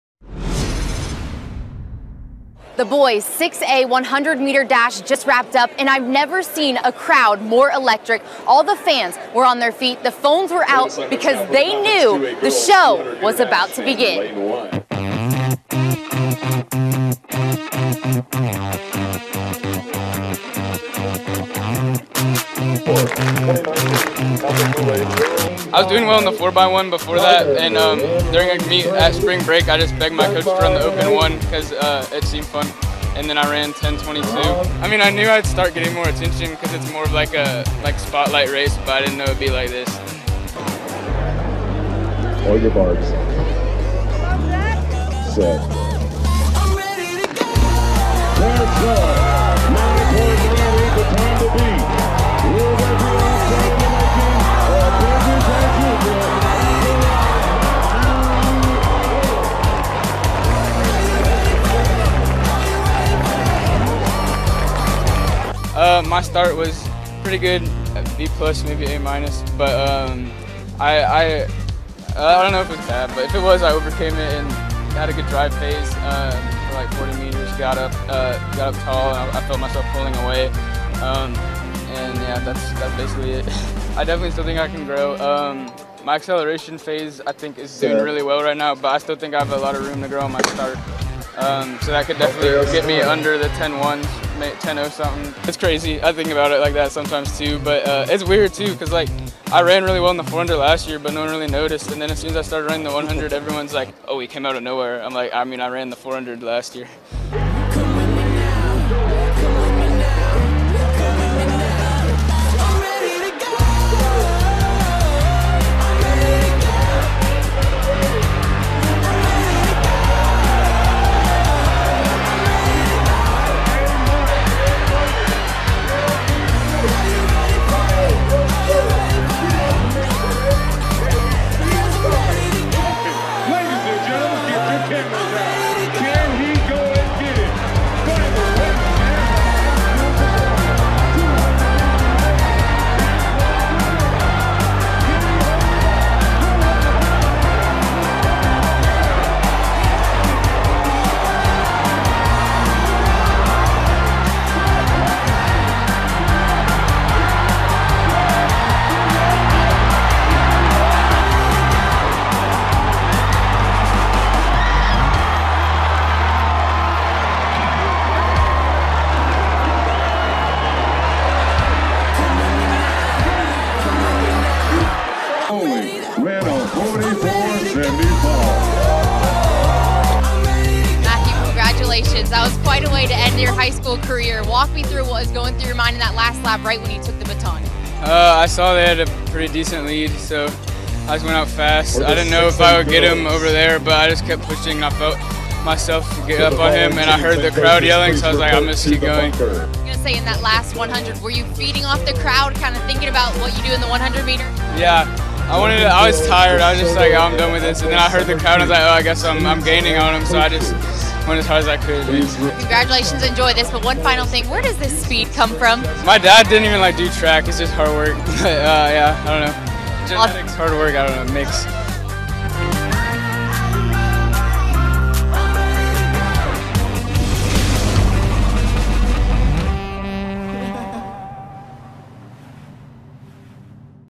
Matthew Boling puts on a show at 2019 UIL Track and Field State Championships
Matthew Boling talks about his dominating performance at the 2019 Texas High School Track & Field Championships. Boling is committed to run Track at the University of Georgia.